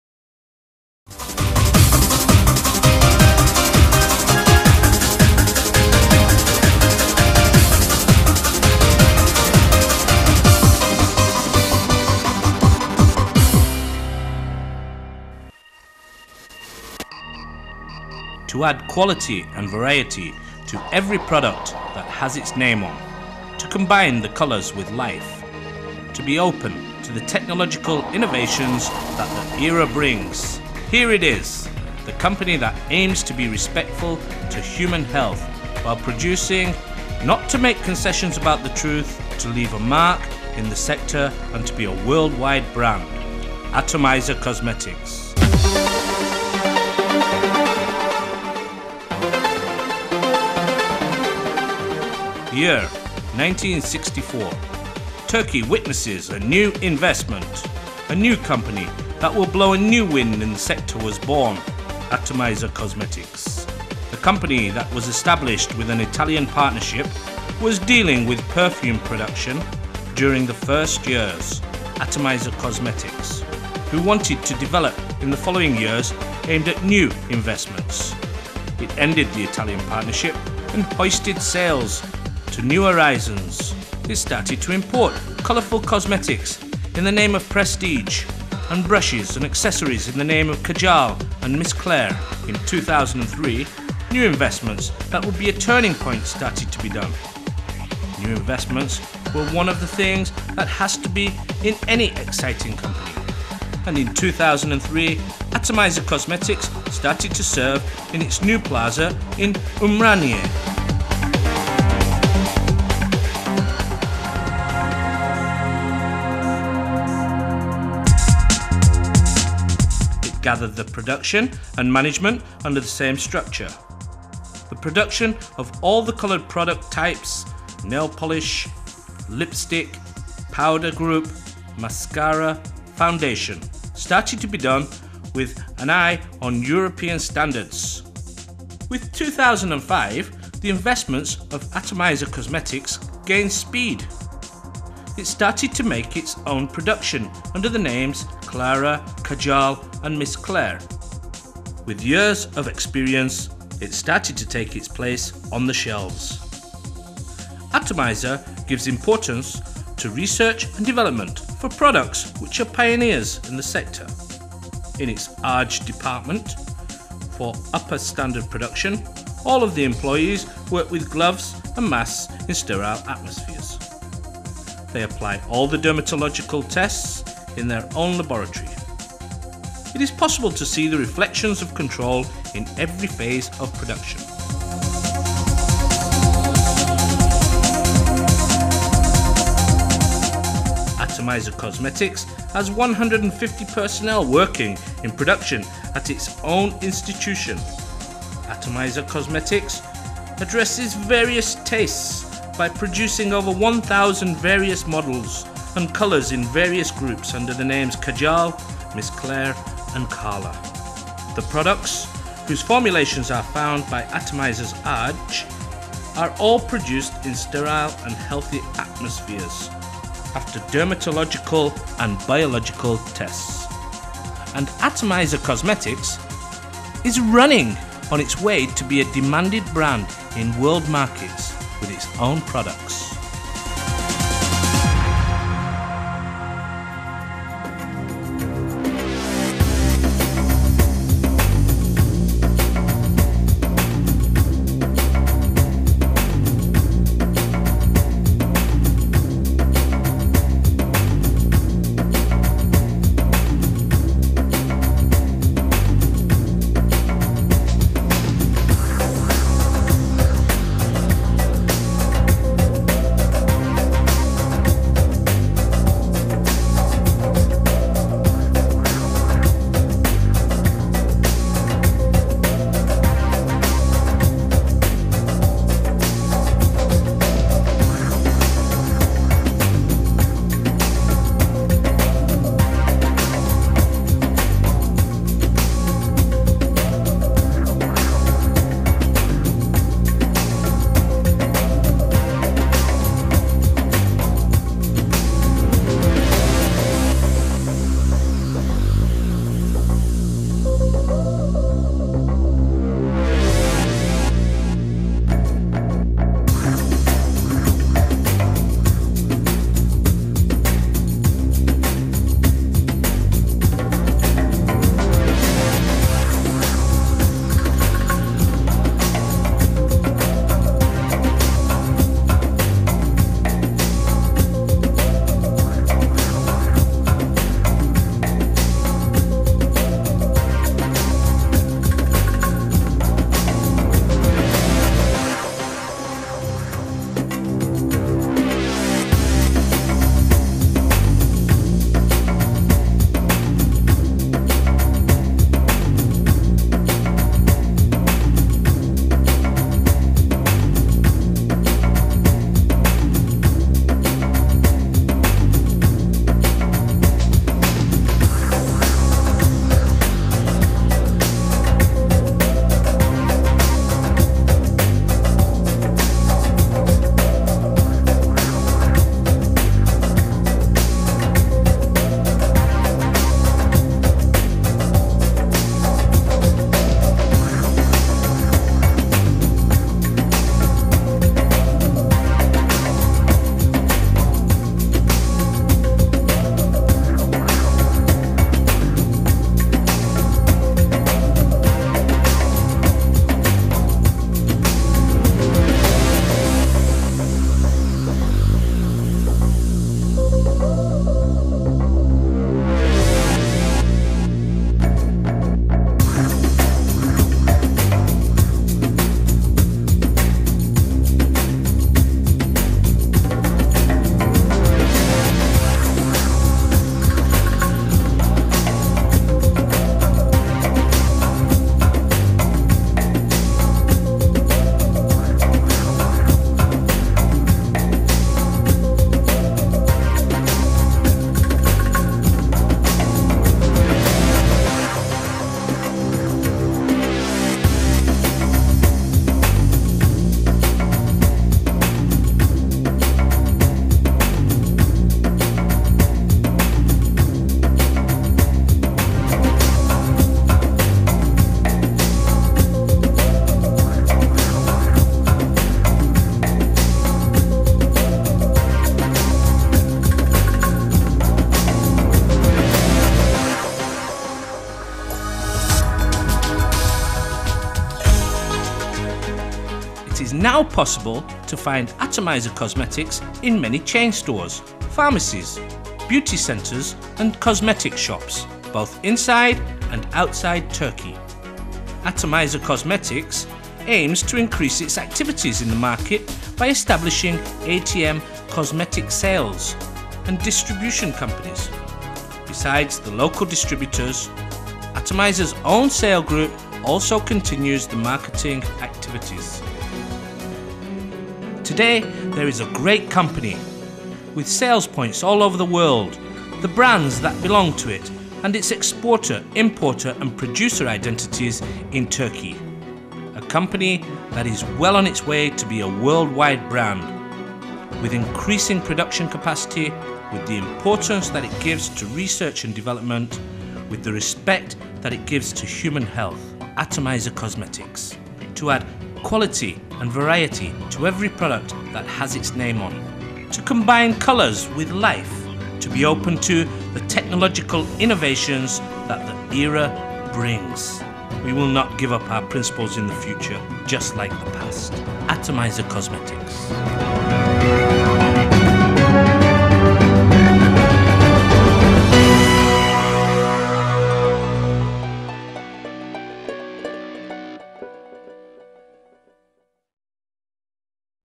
Introduction Video for our manufacturer company Atomizer Kozmetik
Introduction Video for our manufacturer company Atomizer Kozmetik, we manuafcture color cosmetics for private label customers beside our famous brands KAJAL, CARLA and LILIYUM